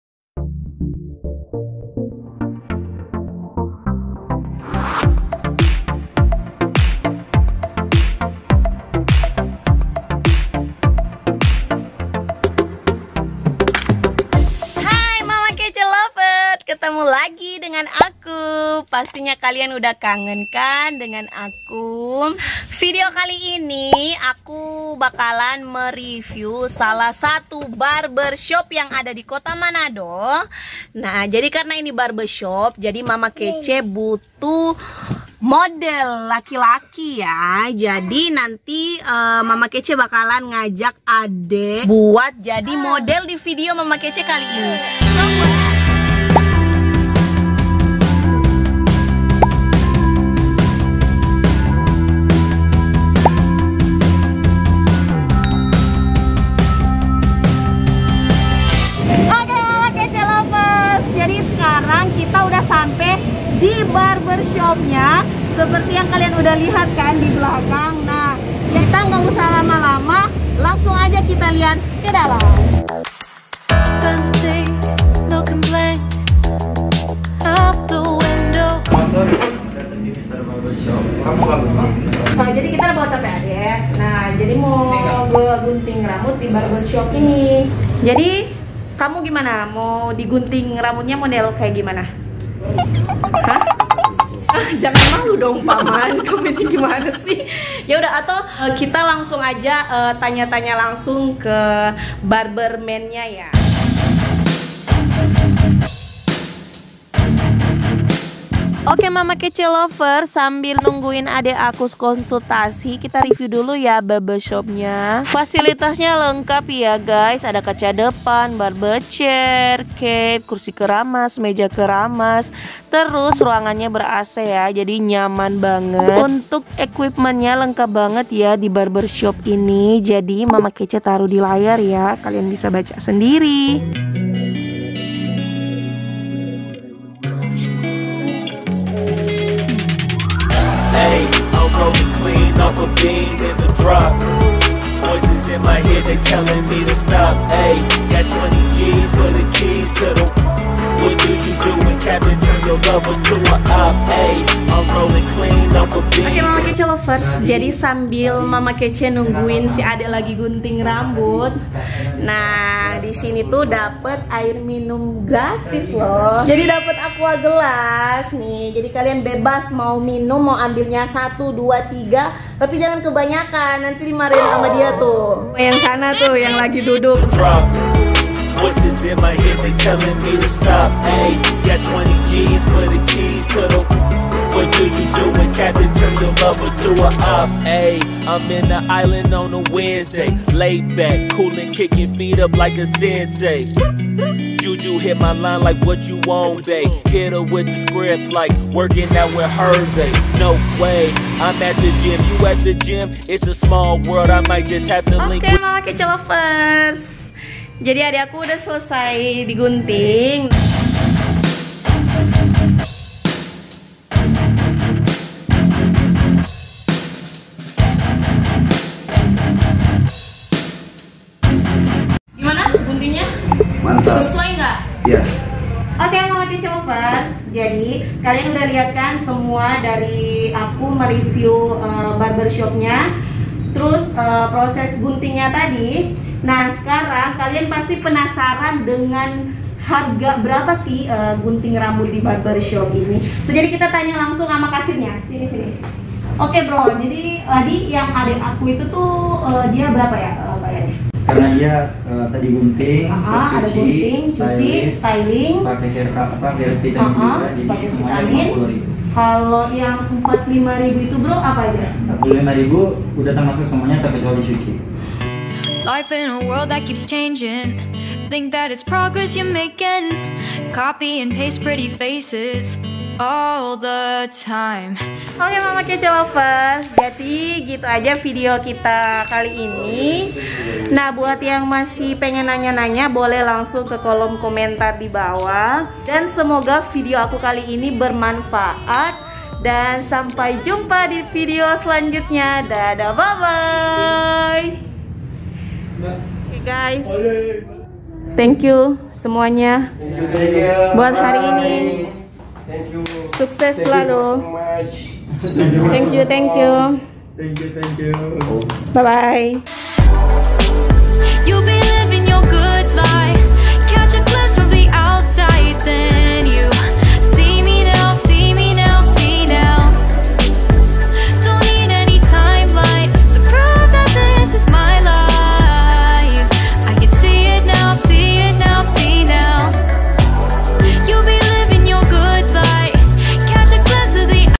UNDERCUT FOR MEN | REVIEW MR BARBERSHOP MANADO
Cuma di barbershop ini !!! Setelah gunting rambut bisa langsung di cuci tanpa pindah tempat, jadi lebih simpel..

Sudah murah, nyaman banget lagi, dapat minuman gratis pulak.. yakin kalian gak mau gunting rambut disini ???

Lokasi : samping pos security kawasan, dibelakang indomaret depan ITC Manado.